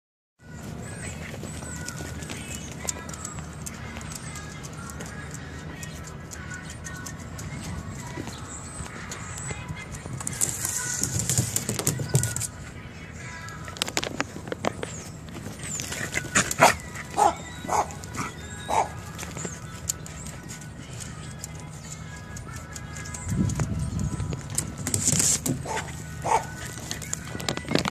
Corgi Slide
Ein or Ruff Rider as dubbed by the Fort Smith's Times Record has many champions in his line. He inspires and warms the hearts of his fans by being a chill little dog as well as a champion ham.